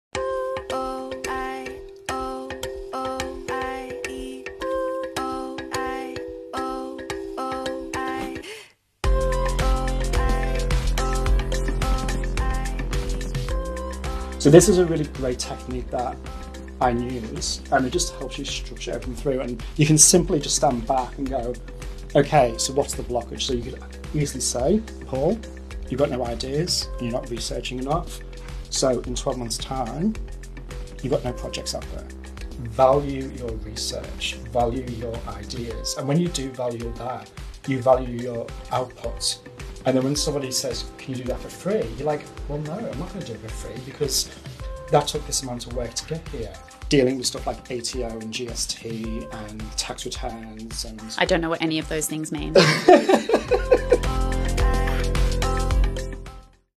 MANAGING YOUR CREATIVE PRACTICE (ON-DEMAND) Trailer
Paul McDonald will share his experience in managing a diverse practice and demanding schedule while keeping creativity at the centre of his efforts. Walking you through his own workspace he will show you techniques and methods he uses to effectively manage projects, deadlines, and work-flow to overcome creative blocks and generate new ideas into meaningful projects and bodies of work.